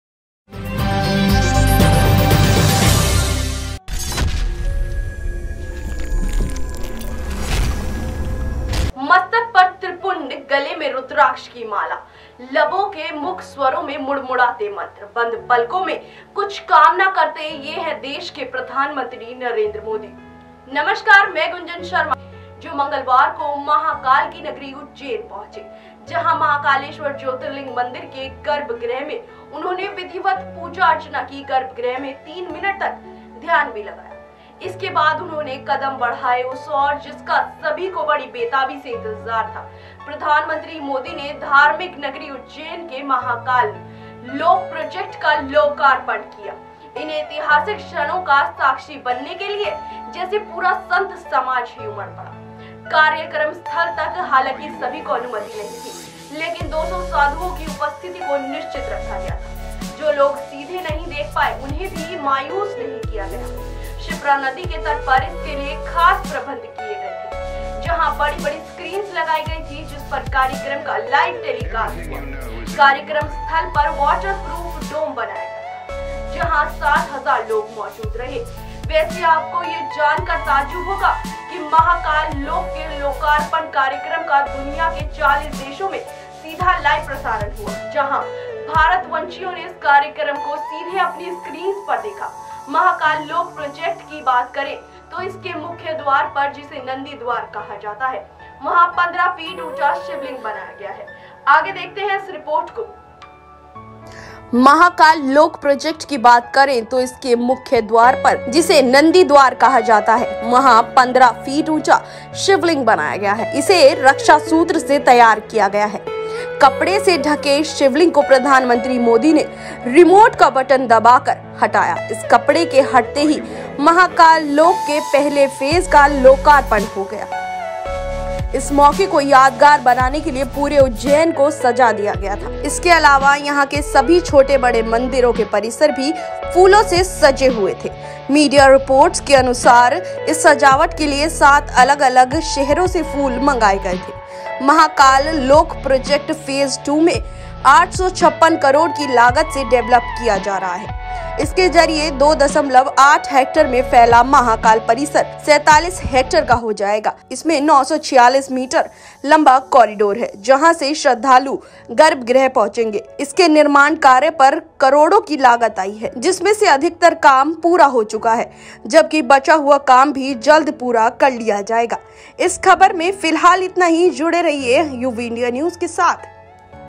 Mahakal Lok Inauguration: PM Modi ने महाकाल लोक का किया लोकार्पण, देखें भव्यता | UVINDIANEWS
प्रधानमंत्री मोदी (PM Modi) (PM Narendra Modi) ने धार्मिक नगरी उज्जैनी में महाकाल लोक प्रोजेक्ट का लोकार्पण (Mahakal Lok Project inaugurated in Ujjain) (Mahakal Lok inauguration) किया (PM Modi inaugurated Mahakal Lok)। इन ऐतिहासिक क्षणों का साक्षी बनने के लिये, जैसे पूरा संत समाज ही उमड़ पड़ा। कार्यक्रम स्थल तक हालांकि सभी को अनुमति नहीं थी, लेकिन 200 साधुओं की उपस्थिति को निश्चित रखा गया था। जो लोग इसे सीधे नहीं देख पाए उन्हें भी मायूस नहीं किया गया। शिप्रा नदी (Shipra River) के तट पर इसके लिए खास प्रबंध किए गए थे। जहां बड़ी-बड़ी स्क्रीन्स लगाई गई थीं, जिस पर पूरे कार्यक्रम का लाइव टेलीकास्ट हुआ। कार्यक्रम स्थल पर वाटर प्रूफ डोम बनाया गया था, जहां 60 हजार लोग मौजूद रहे। वैसे आपको ये जानकर ताज्जुब होगा कि महाकाल लोक (Mahakal Lok) के लोकार्पण कार्यक्रम का, दुनिया के 40 देशों में सीधा प्रसारण हुआ। जहां भारतवंशियों ने इस कार्यक्रम को सीधे अपनी स्क्रीन्स पर देखा।